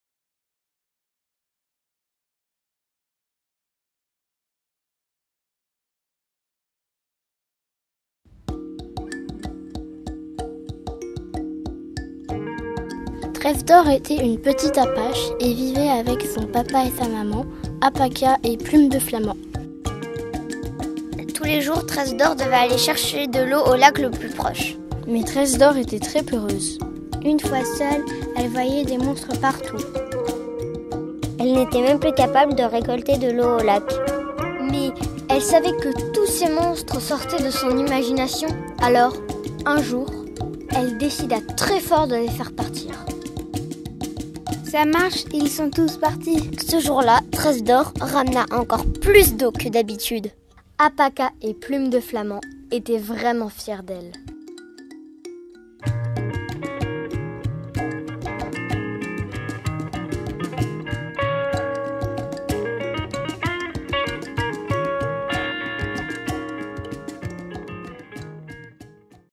[KLAP KLAP 2019] peur pliable (réalisé par les 9-12 ans)
Animation réalisé par les 9-12 ans lors du festival Klap Klap 2019.